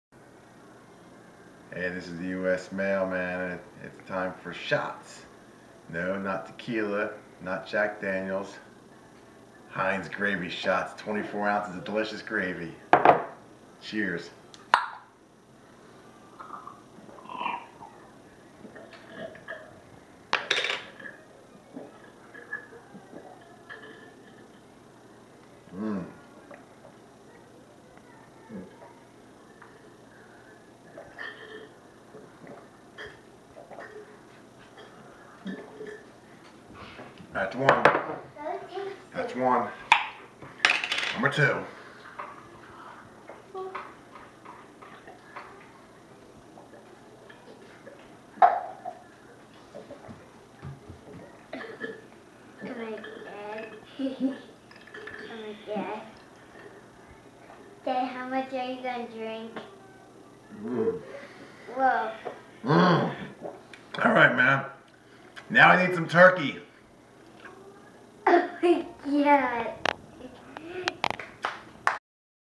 Turkey Gravy Chug
Was sitting around and just a little thirsty so why not have a few 12 oz gravy shots to quench that thirst. Down the hatch in 47 seconds.